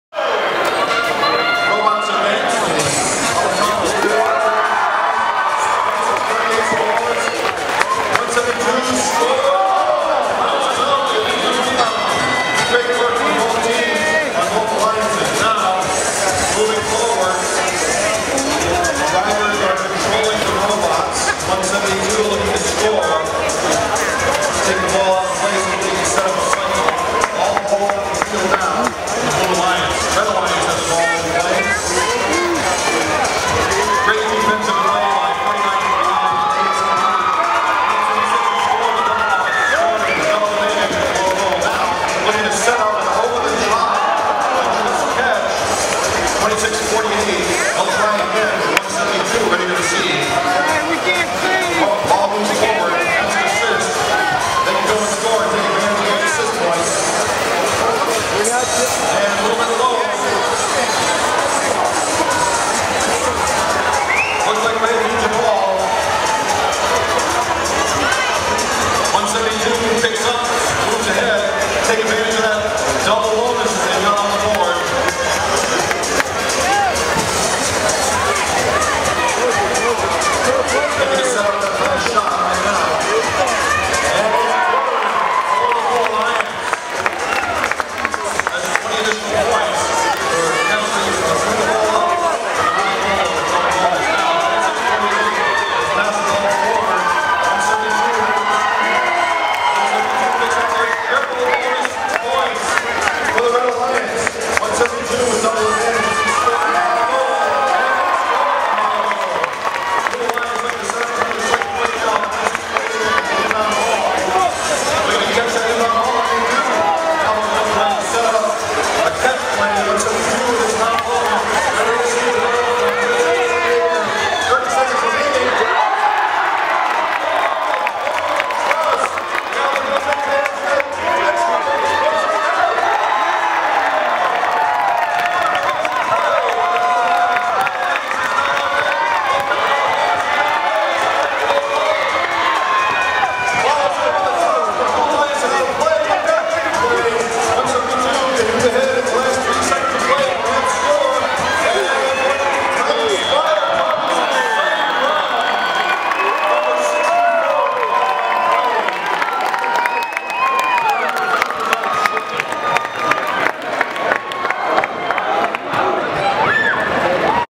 GSD Qualifying Match 64
FIRST Team 172 competes at Granite State District Qualification Matches in Nashua, NH